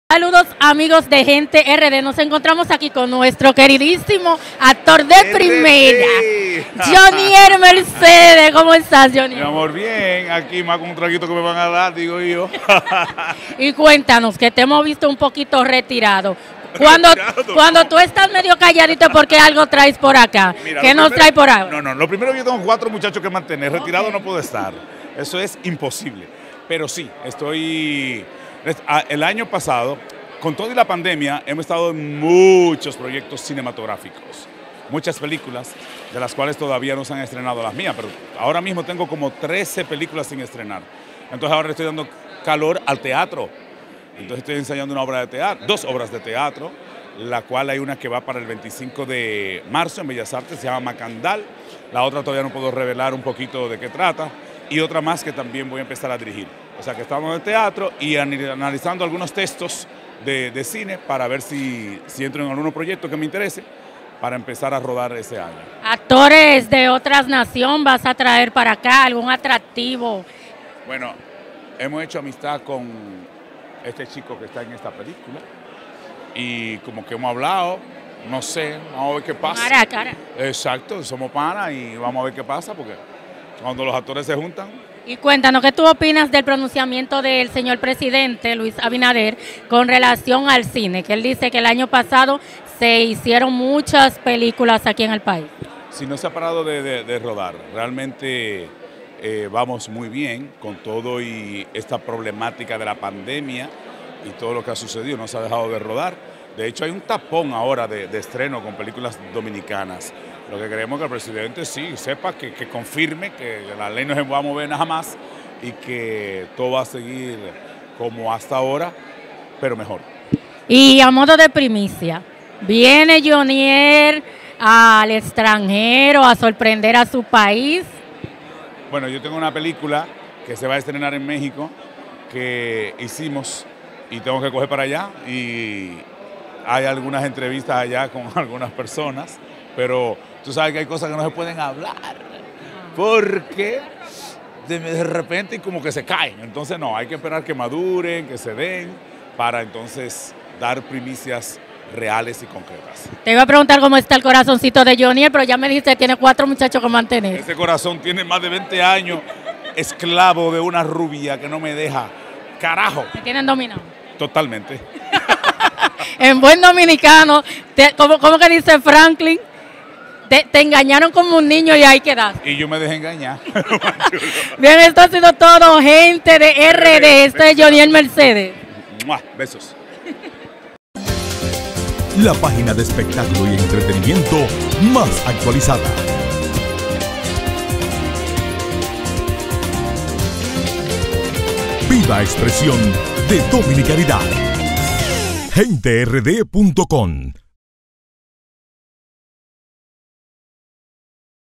Johnnie Mercedes llega al teatro con 13 películas por estrenar
Recuerda seguir este canal y si te gusta el vídeo dejános saber y comparte